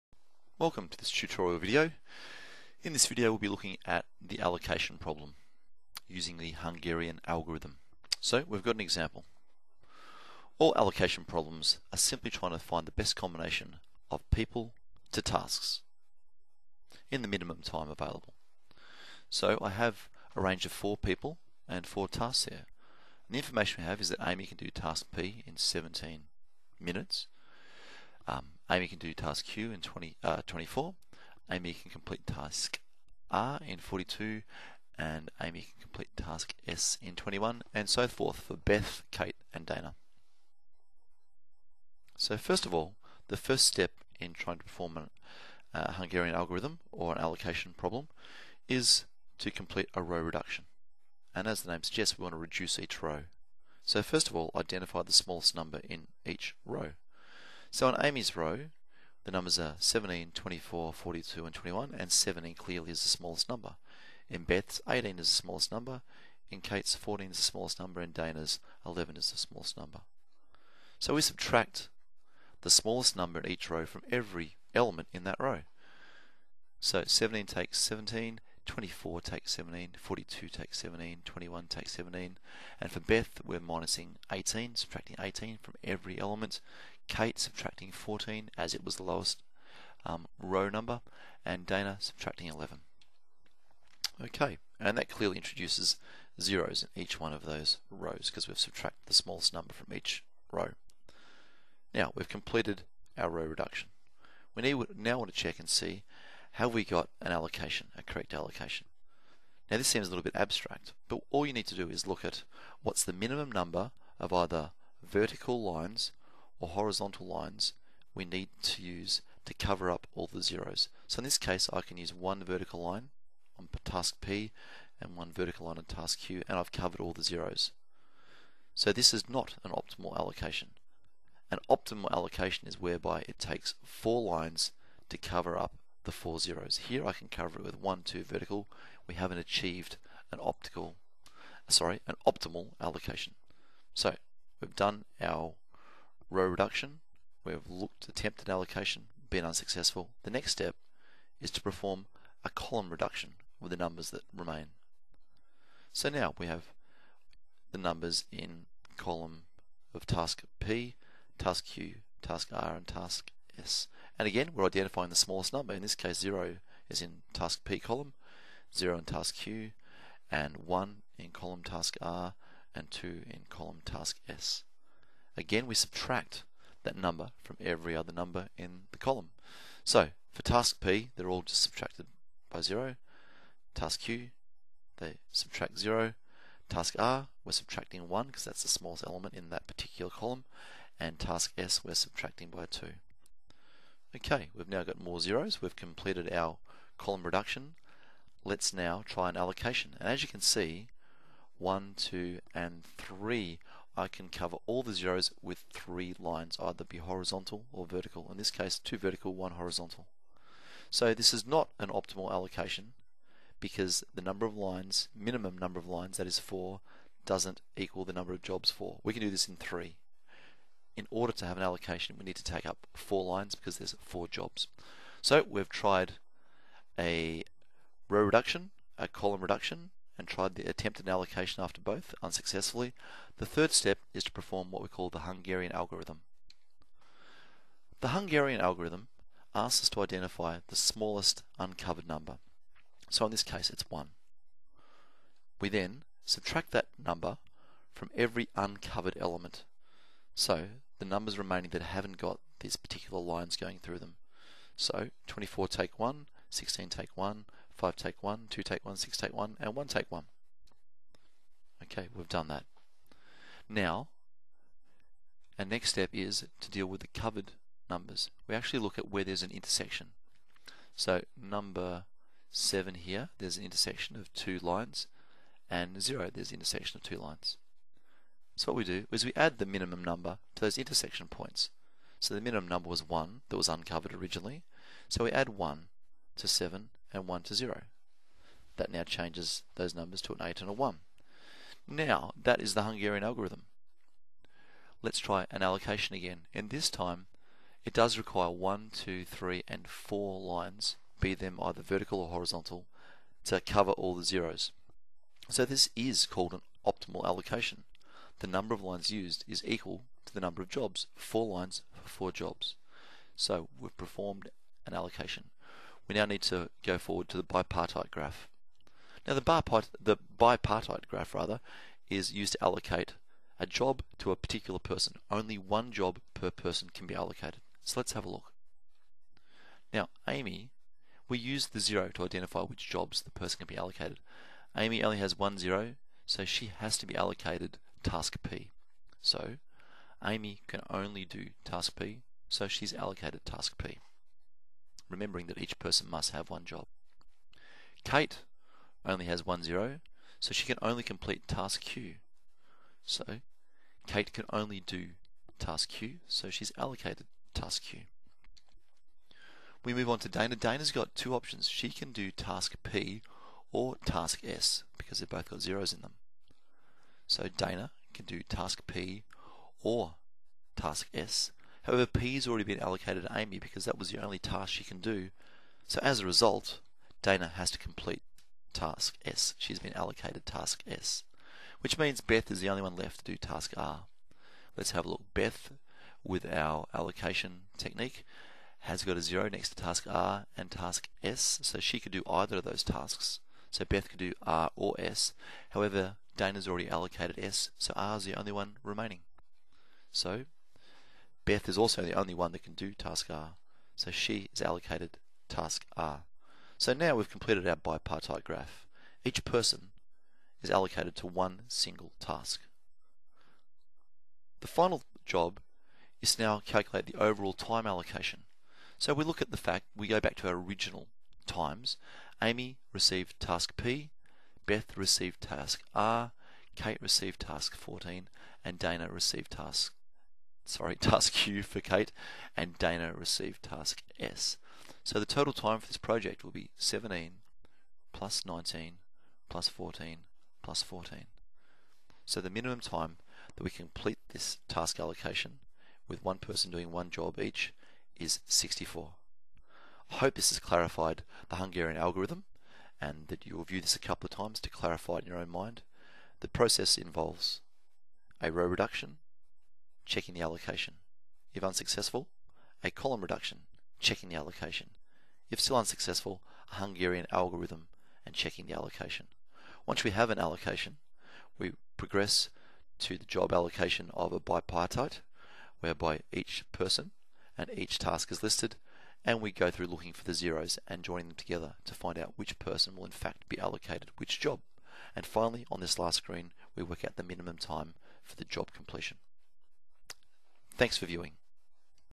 The Allocation Problem 1
This video provides a simple example of the allocation problem, using row reduction, column reduction and the Hungarian Algorithm.  It is useful to VCE Further Maths students studying the Networks and Decision Making module.